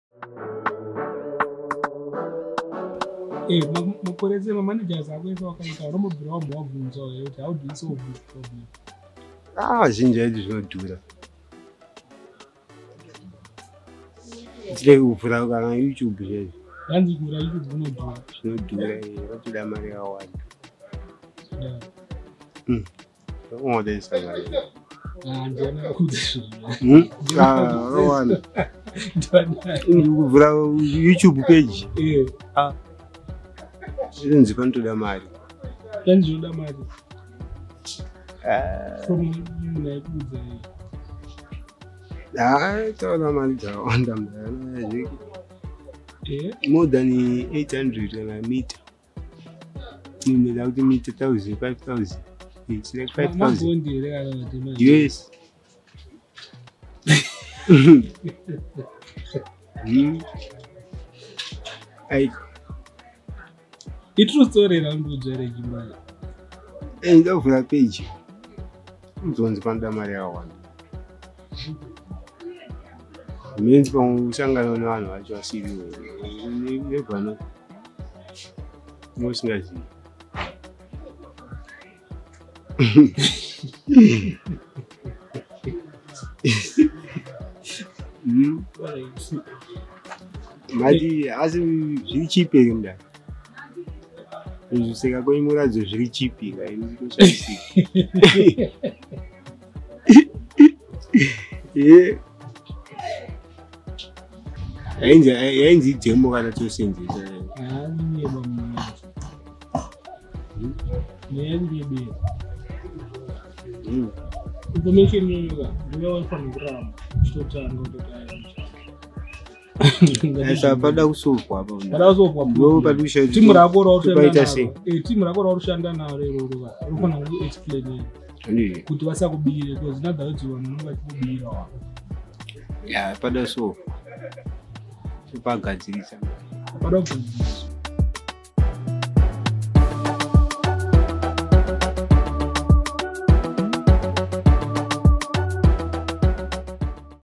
Zimdancehall Stories (Shorts) G MBARE
G Mbare talks about how hard it was for him to open a You Tube Account, and how everyone around him made him believe it was very expensive